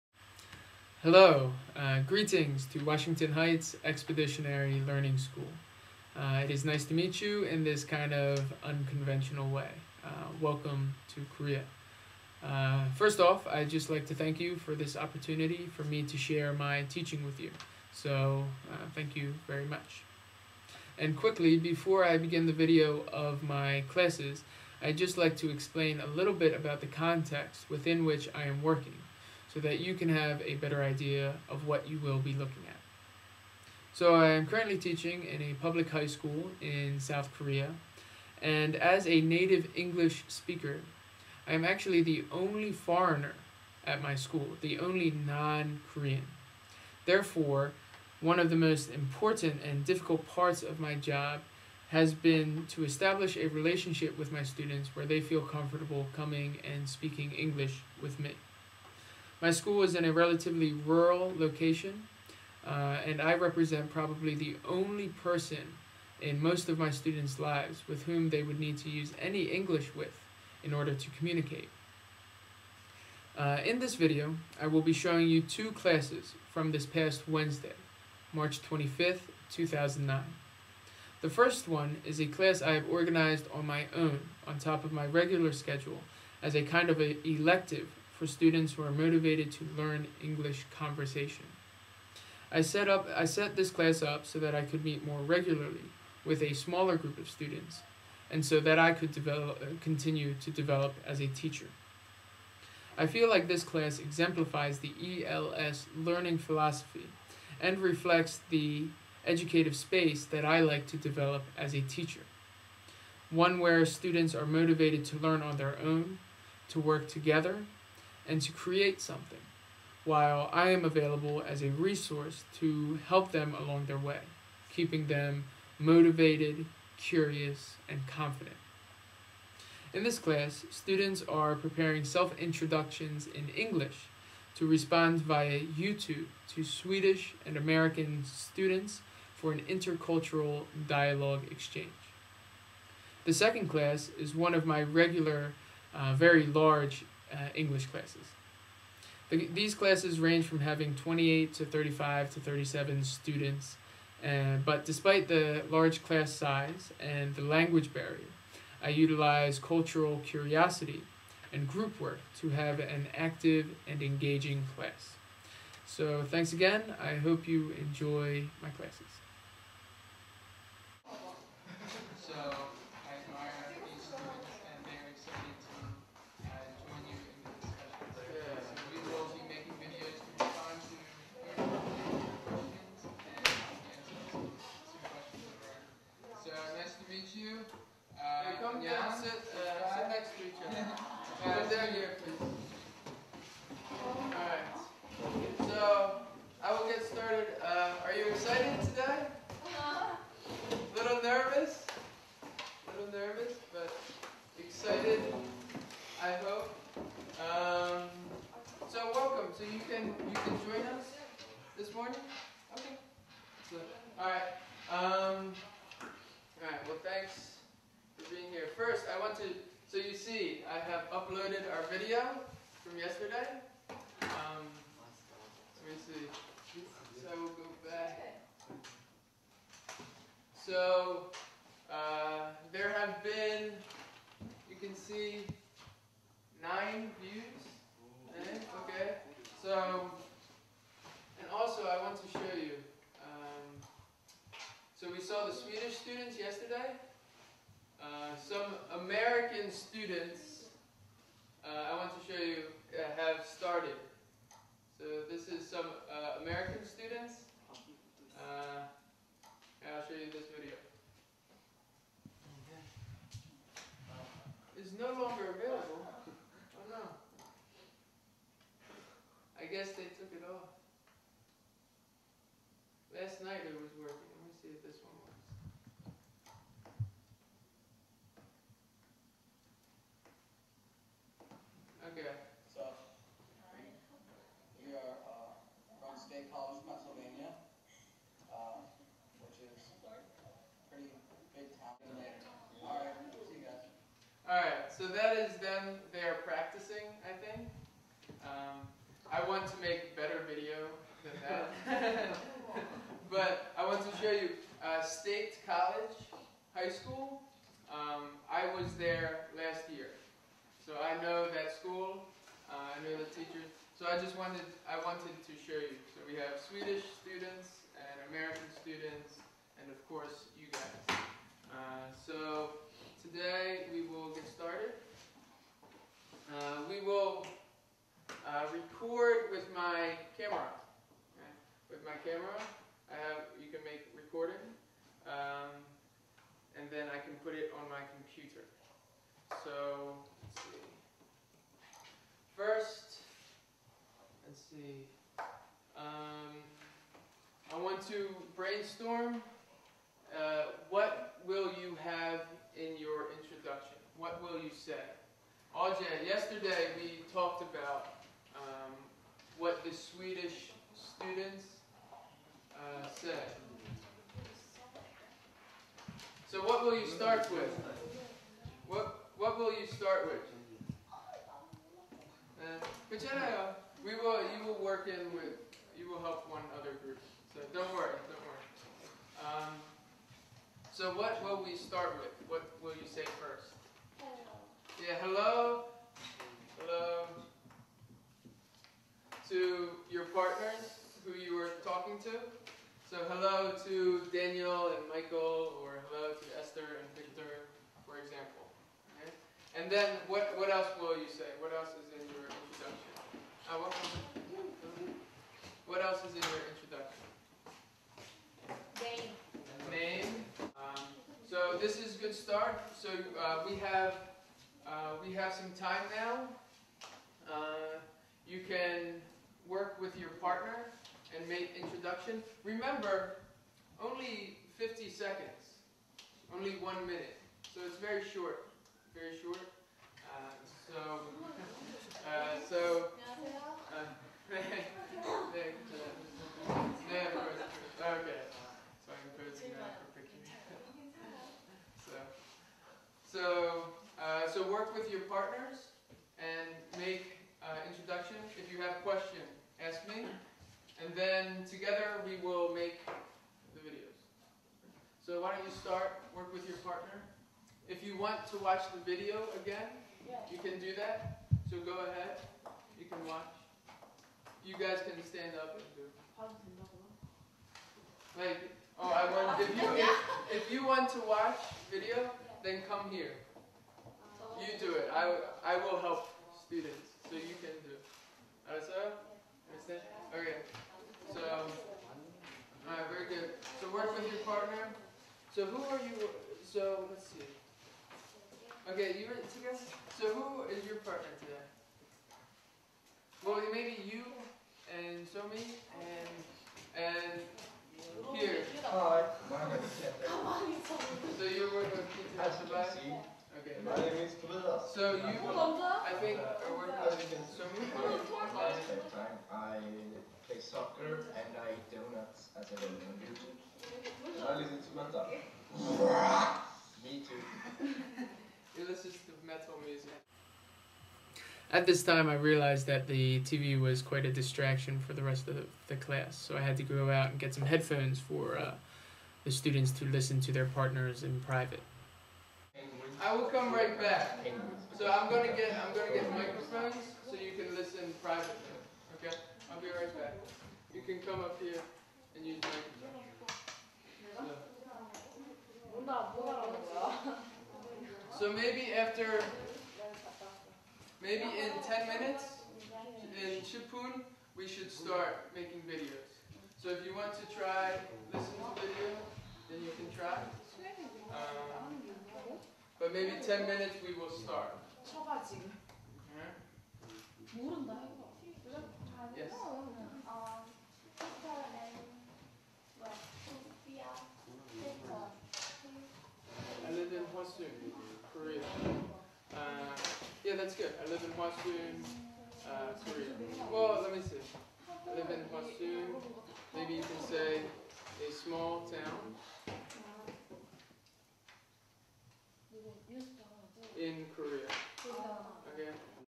Luke Hwasun Teaching Video 1
Video of me teaching in Korea.